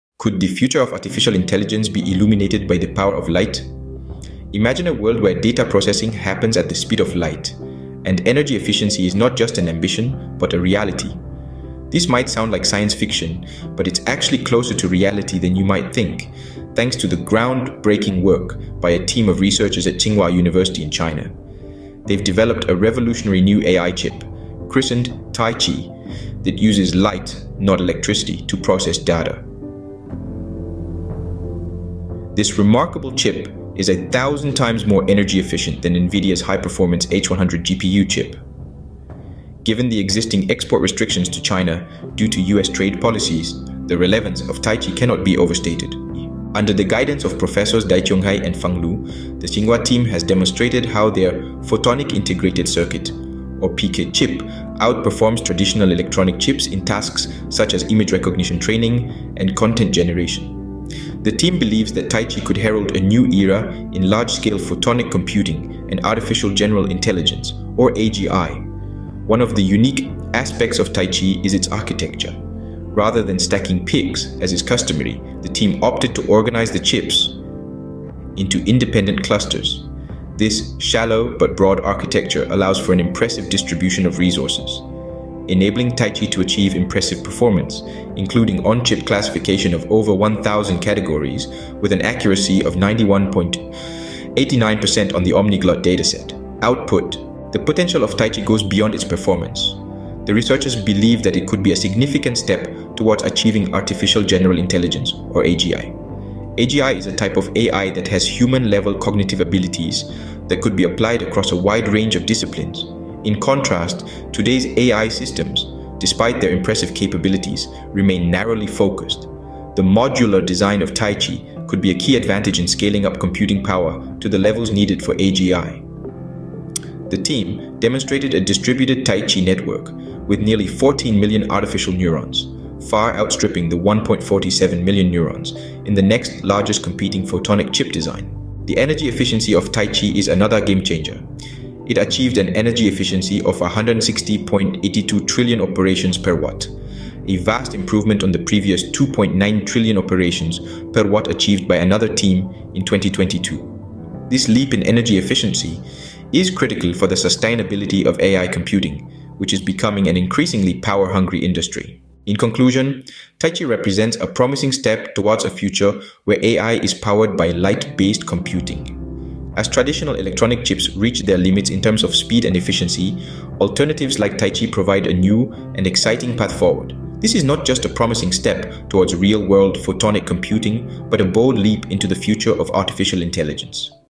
Taichi Photonic Chip to Revolutionise Large Scale AI Computing
Researchers at Tsinghua University in China have developed a revolutionary new artificial intelligence (AI) chip that uses light instead of electricity to process data.

Dubbed “Taichi,” the chip is reportedly over 1,000 times more energy-efficient than Nvidia’s high-performance H100 GPU chip. Taichi is especially relevant given export restrictions to China due to US trade policies.

Led by professors Dai Qionghai and Fang Lu, the Tsinghua team explained how their photonic integrated circuit (PIC) chip outperforms traditional electronic chips in tasks like image recognition training and content generation.

“Taichi paves the way for large-scale photonic computing and advanced tasks, further exploiting the flexibility and potential of photonics for modern artificial general intelligence (AGI),” the researchers stated in their paper.

Unlike conventional chip approaches that stack PICs, the Taichi team opted for an innovative architecture. They organized the chips into independent clusters, creating a shallow but broad architecture.

This distribution of resources allows Taichi to achieve impressive performance. Performance figures include on-chip classification of over 1,000 categories with 91.89% accuracy on the Omniglot dataset.

The Omniglot dataset is widely used to benchmark machine learning models. It contains a diverse collection of handwritten characters from over 50 different writing systems.

“It was not an exclusive algorithm only for Taichi. The computation and task distributing could also help existing PICs to extend their computing capacity for more advanced tasks,” the team explained.

Towards artificial general intelligence
According to the researchers, Taichi could potentially make artificial general intelligence (AGI) a reality. AGI is a form of AI with human-level cognitive abilities that could be applied across a wide range of disciplines. In comparison, despite their capability, current systems remain narrow in focus.

“We anticipate that Taichi will accelerate the development of more powerful optical solutions as critical support for the foundation model and a new era of AGI,” the researchers stated.

Taichi’s modular design could be a key advantage in scaling up computing power to the levels needed for AGI. The Tsinghua team demonstrated a distributed Taichi network with 13.96 million artificial neurons, exceeding the 1.47 million neurons in the next largest competing photonic chip design.

Taichi achieved an energy efficiency of 160.82 trillion operations per watt (TOPS/W), dwarfing the previous 2.9 TOPS/W achieved by another team in 2022, LiveScience reports. This dramatic improvement in energy efficiency is crucial for the sustainability of AI computing, which has become an increasingly power-hungry industry.

A promising path ahead
Taichi hints at a future where artificial intelligence is powered by light-based computing. With traditional electronic chips approaching their limit in terms of speed and efficiency, there is a need for viable alternatives.

“The present work is a promising step toward real-world photonic computing, supporting various applications in AI,”